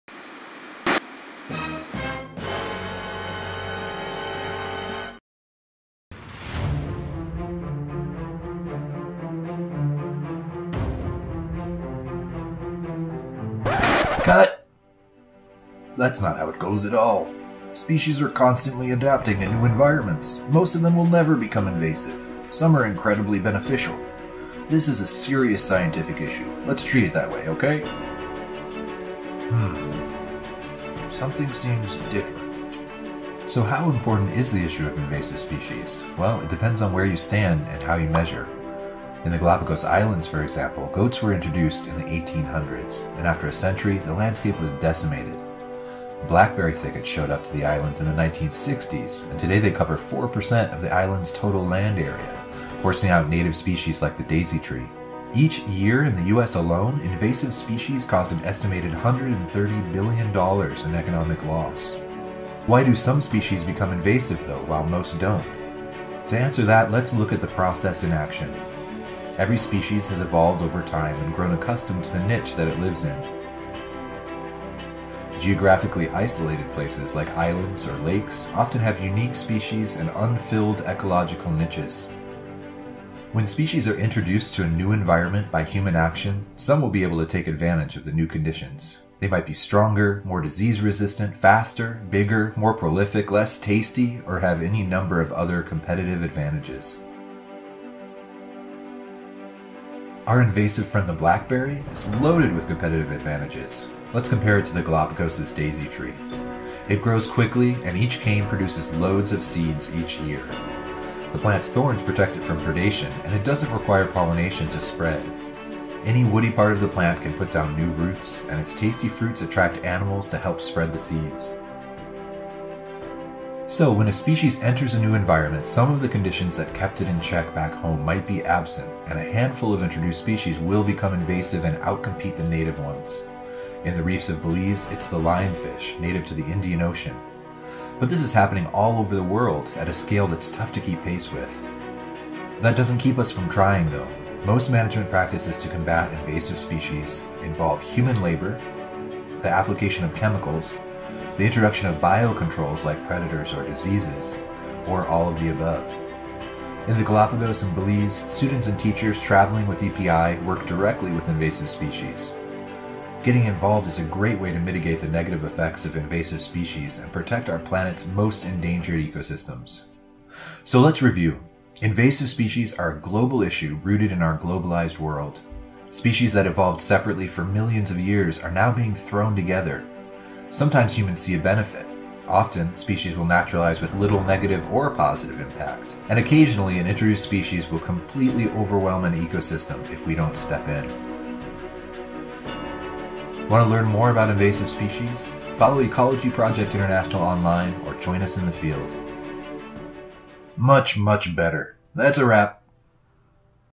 Invasive Species: What Can You Do?
Invasive species are a global issue, causing billions of dollars in economic loss each year. What does that mean for you?

Learn more about what causes a species to become invasive, how humans have been fighting this battle for centuries, and what EPI is doing about it in Belize and the Galapagos Islands.